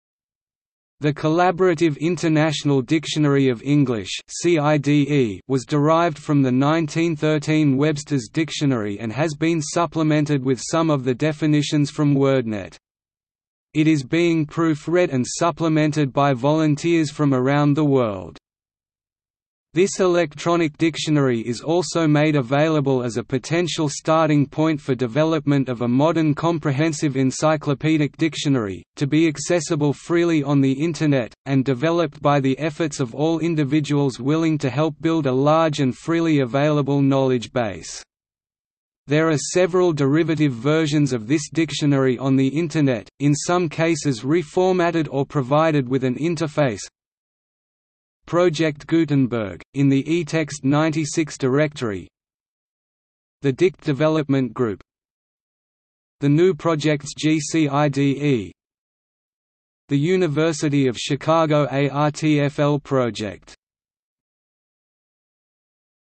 Collaborative International Dictionary of English | Wikipedia audio article
- Socrates

SUMMARY
The Collaborative International Dictionary of English (CIDE) was derived from the 1913 Webster's Dictionary and has been supplemented with some of the definitions from WordNet. It is being proof-read and supplemented by volunteers from around the world.
This electronic dictionary is also made available as a potential starting point for development of a modern comprehensive encyclopedic dictionary, to be accessible freely on the Internet, and developed by the efforts of all individuals willing to help build a large and freely available knowledge base.There are several derivative versions of this dictionary on the Internet, in some cases reformatted or provided with an interface:

Project Gutenberg, in the etext96 directory
The DICT development group
The GNU project's GCIDE
The University of Chicago ARTFL Project